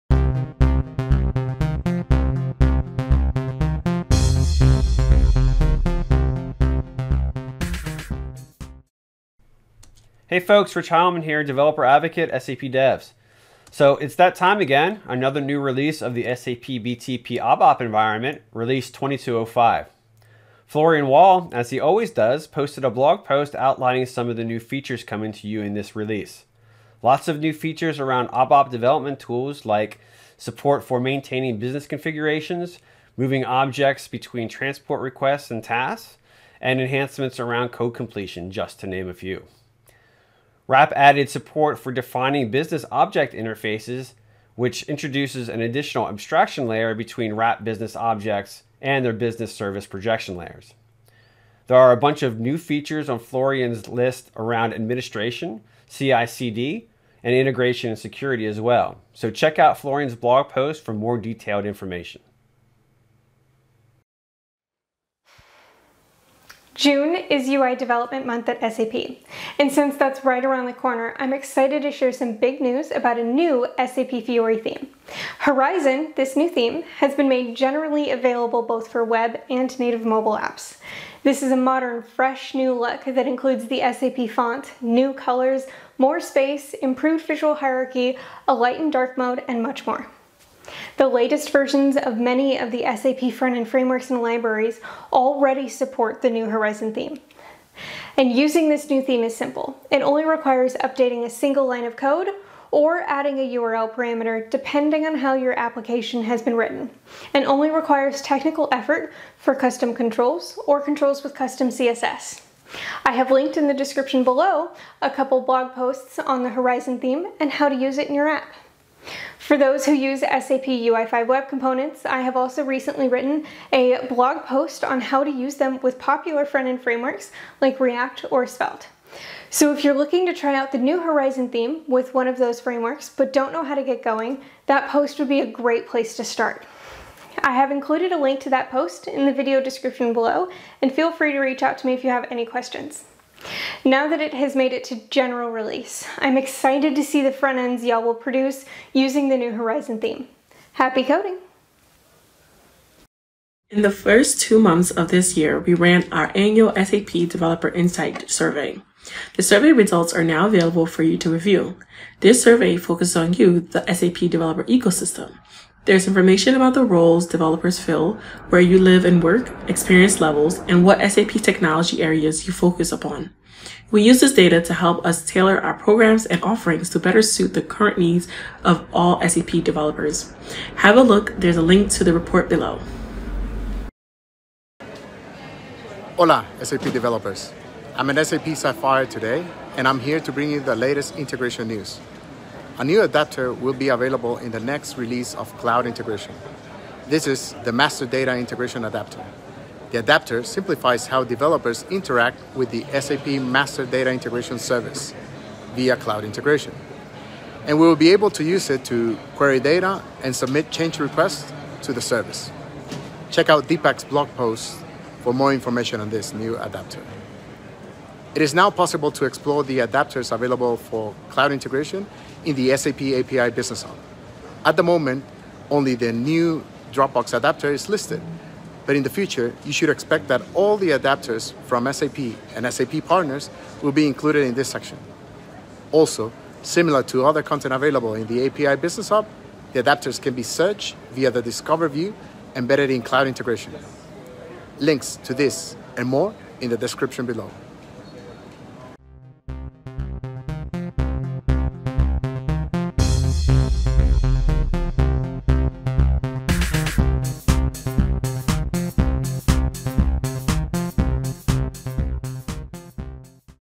ABAP 2205, Horizon Theme, Developer Insights Survey, Integration Adapters | SAP Developer News
SAP BTP, ABAP Environment – Release 2205

Horizon Theme of SAP Fiori Now Generally Available
- Horizon theme of SAP Fiori: update on productive usage for web applications (blog post)
- SAP Fiori Evolution: Horizon Now Generally Available in UI Technologies and Some Products (blog post)

Integration Adapters in SAP API Business Hub
- SAP Integration Suite – Integration with SAP Master Data Integration (MDI) service (blog post)
- Integration Adapters in SAP API Business Hub for SAP Integration Suite (blog post)

0:00 Intro
0:09 ABAP 2205 Release
1:05 Horizon Theme GA
2:32 Developer Insights Survey
3:04 Integration Adapters